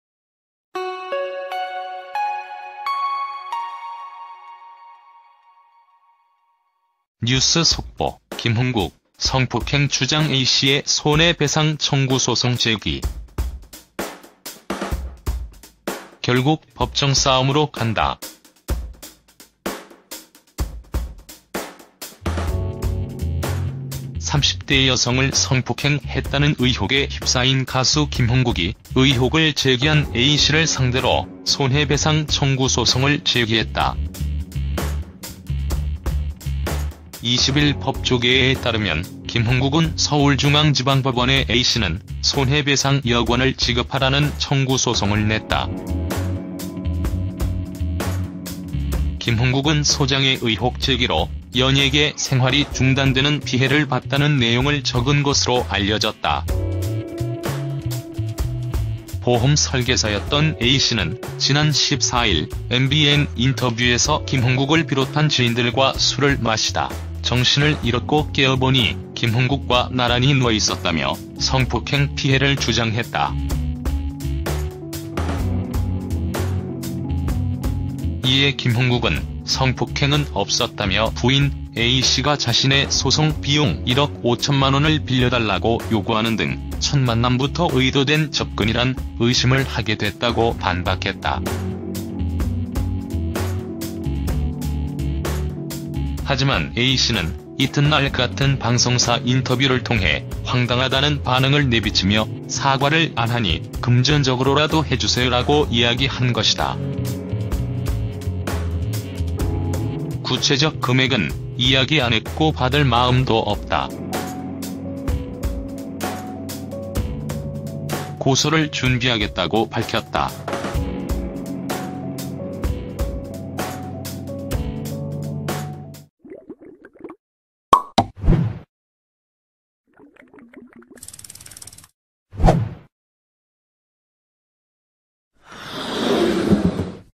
[뉴스 속보],...김흥국, 성폭행 주장 A씨에 손해배상청구 소송 제기.
20일 법조계에 따르면 김흥국은 서울중앙지방법원에 'A씨는 손해배상 2억원을 지급하라'는 청구 소송을 냈다. 김흥국은 소장에 "의혹 제기로 연예계 생활이 중단되는 피해를 봤다"는 내용을 적은 것으로 알려졌다.